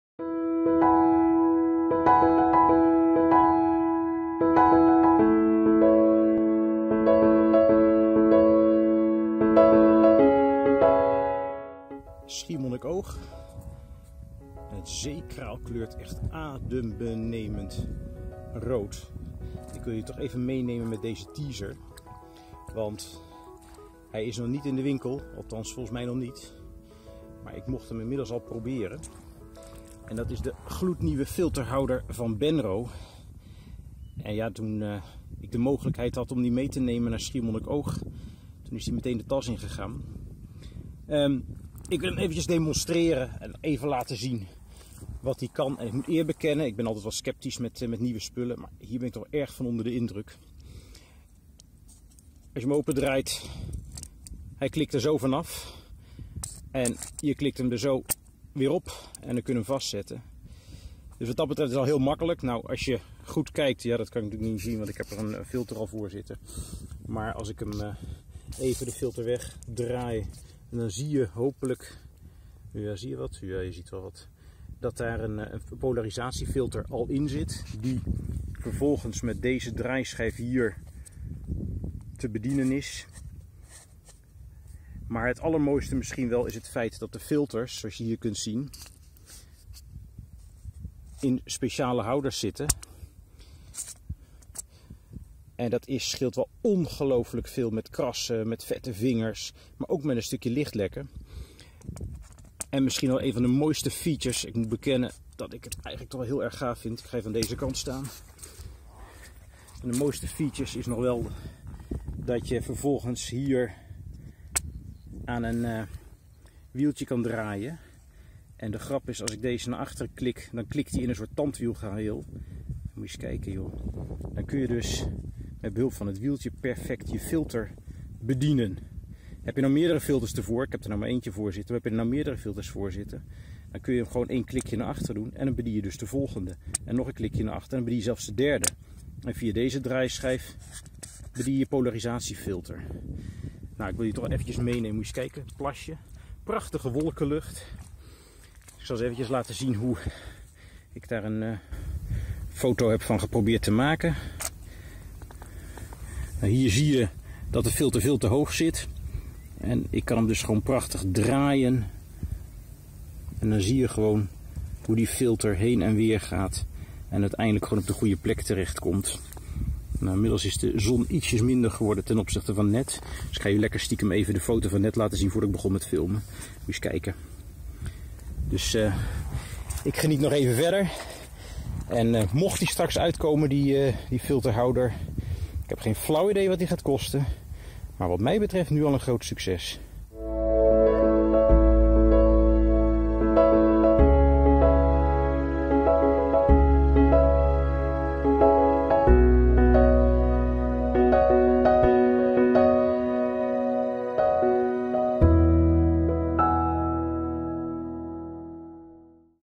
Op Schiermonnikoog met de nieuwe Benro FH100M2 filterhouder
In september mocht ik van Benro Benelux al spelen met de nieuwe filterhouder FH100M2 waarin de 100mm filters niet rechtstreeks geplaatst worden maar middels diaraampjes. Via een ingenieus tandwielsysteem kun je dan de filters perfect op de goede hoogte plaatsen. Daarnaast kun je ook nog steeds de nieuwe polarisatie filter via de achterkant van de houder bedienen. Reden om hem eens uit te testen op het rode rif van Schiermonnikoog...